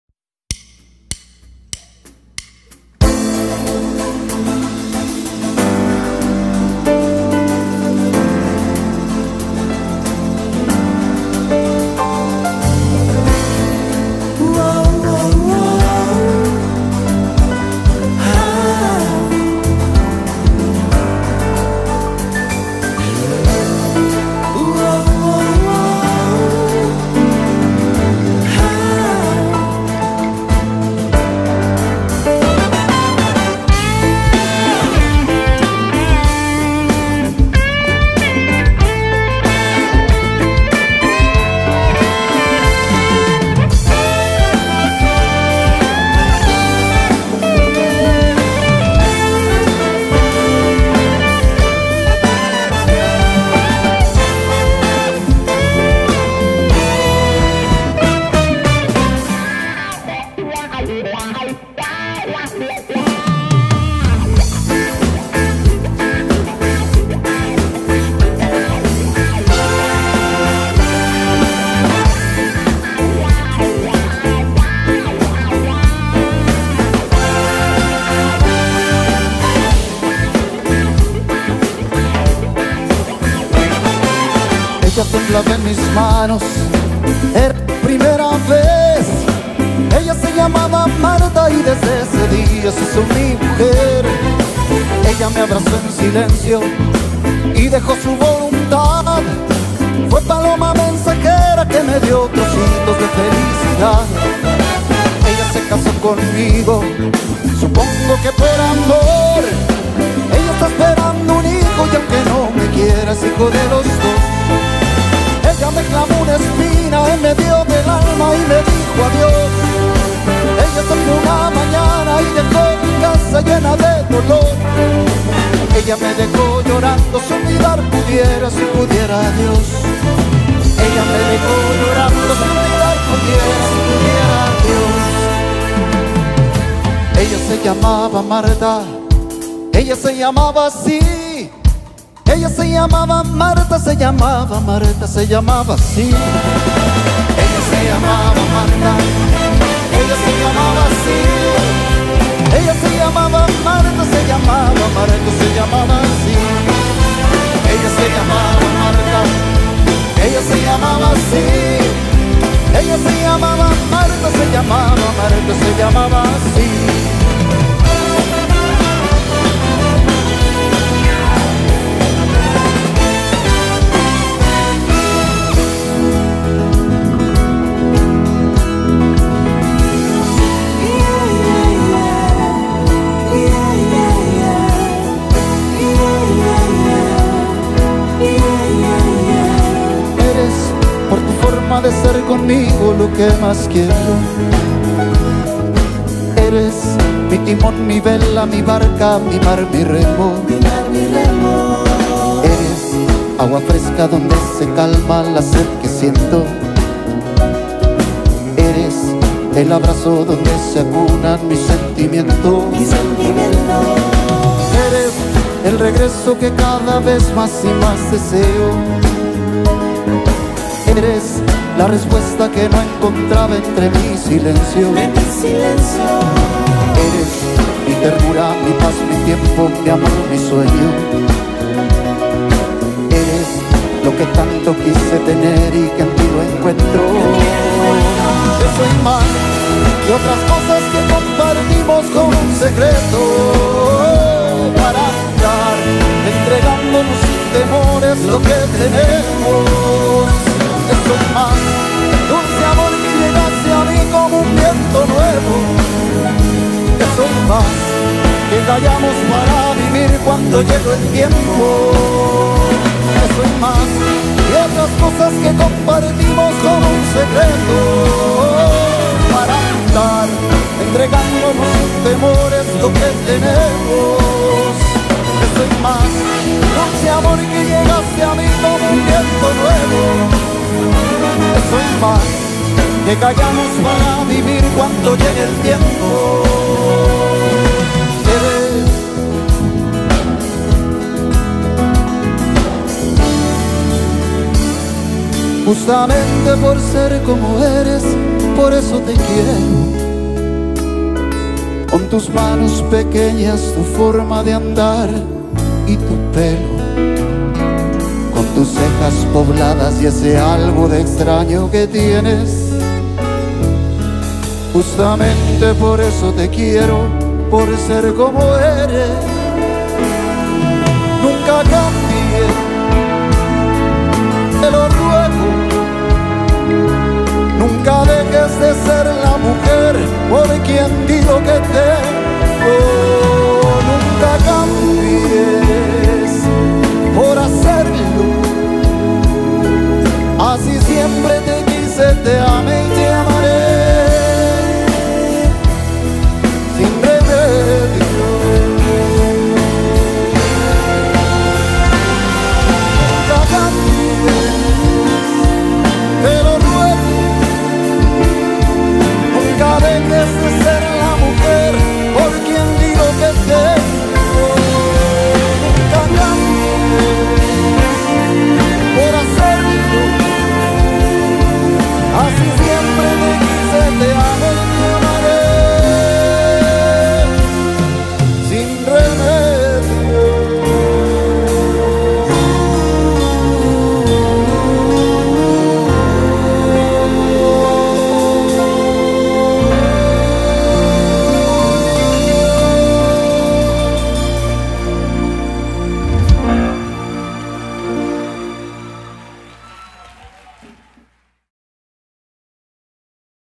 Concierto Homenaje a 2 Grandes (Obertura)
Concierto Homenaje a 2 Grandes (Obertura) a José María Napoleón y Marco Antonio Solís.